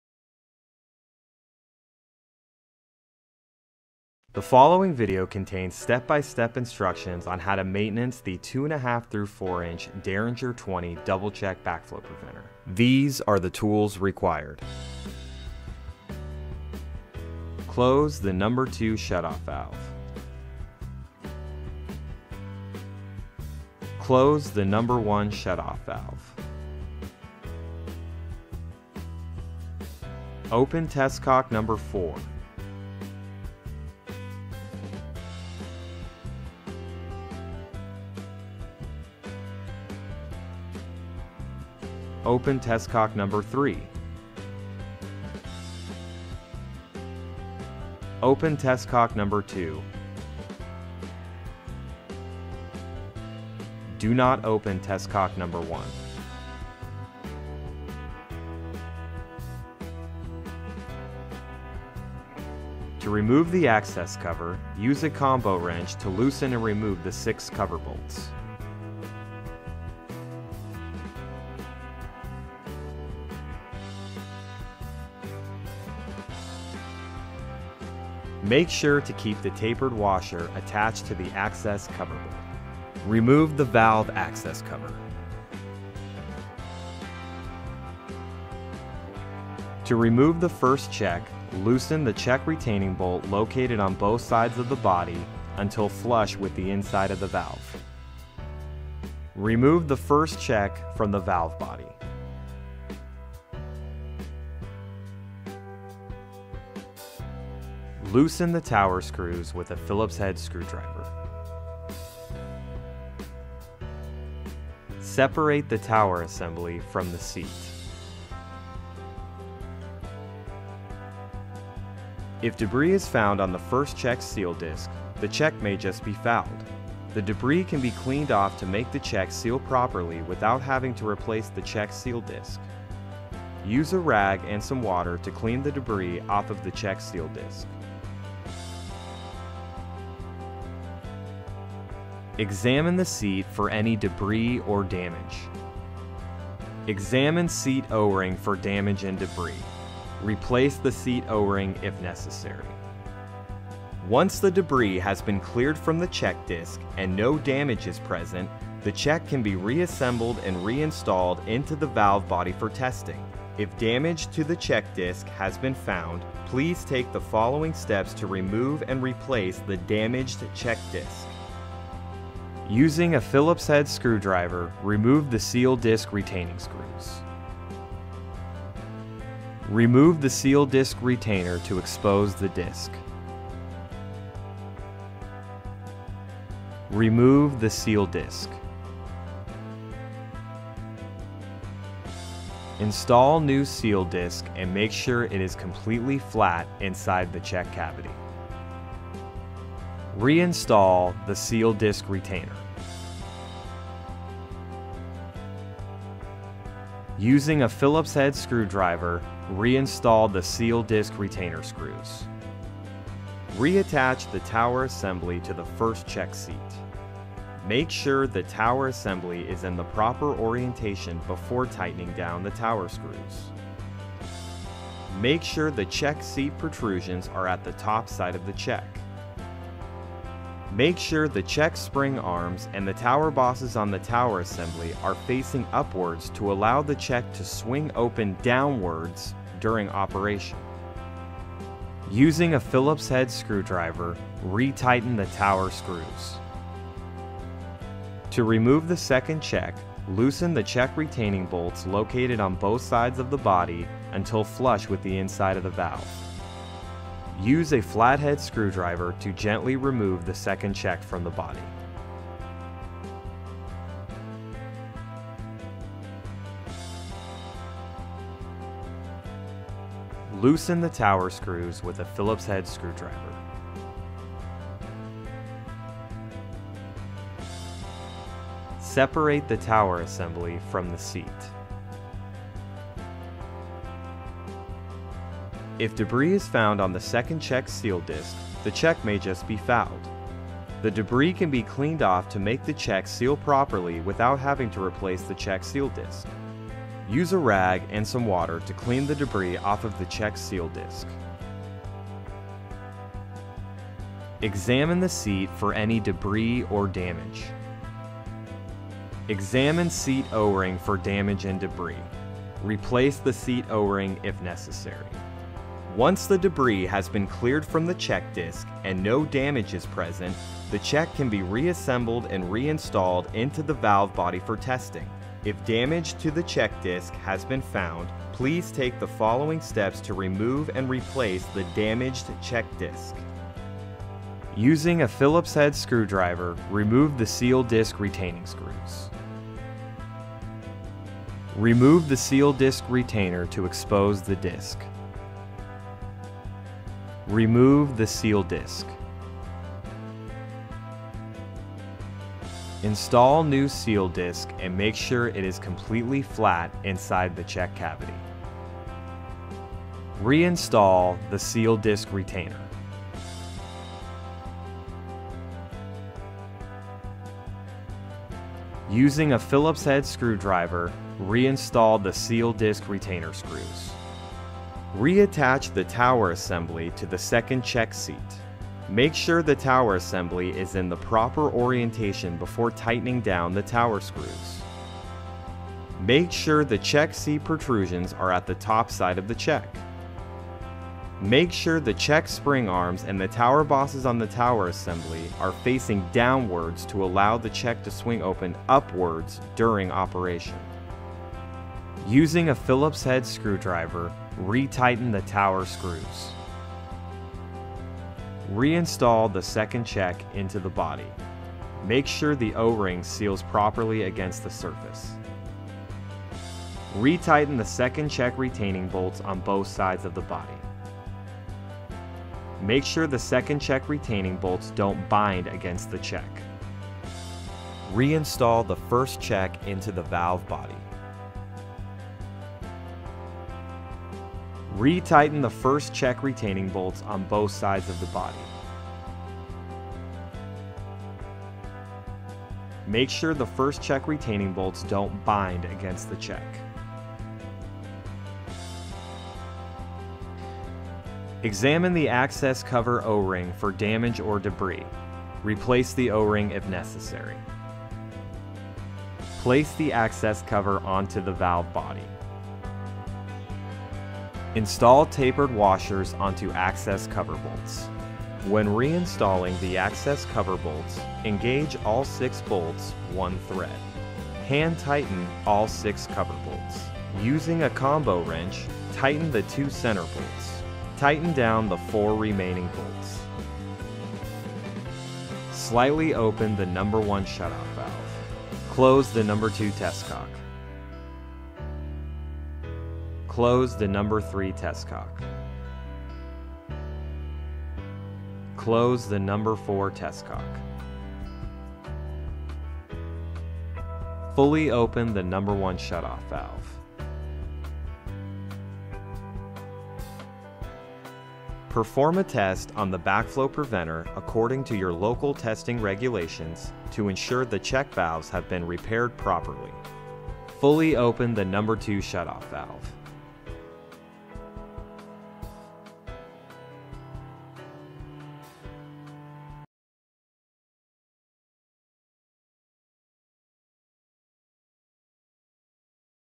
Ames Deringer Model 20 2 ½" – 4" Double Check Valve Assembly Repair Video
How to repair and rebuild an Ames Deringer Model 20 2 ½" – 4" backflow preventer.